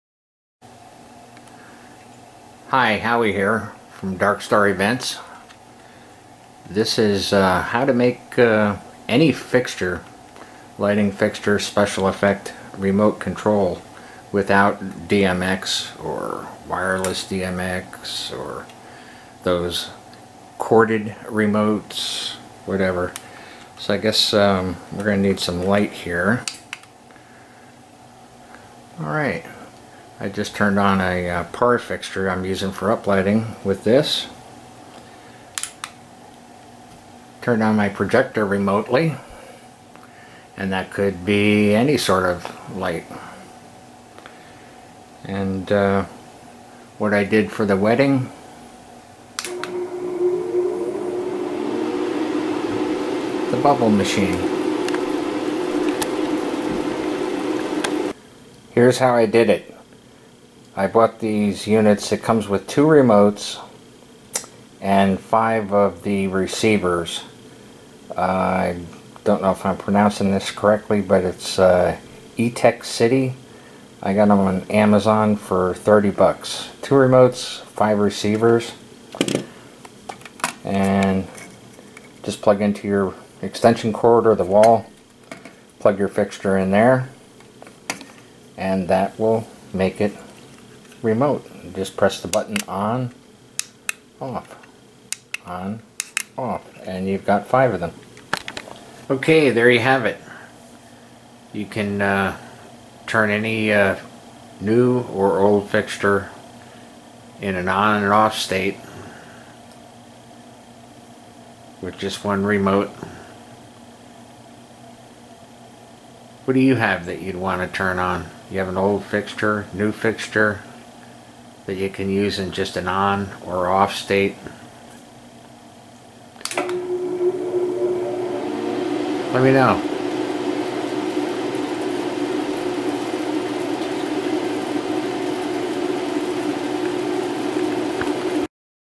Mobile DJ remote control for lighting & special effects
Control fixtures and special effects to an off or on state without DMX  long cable runs or wireless DMX transmitters for $30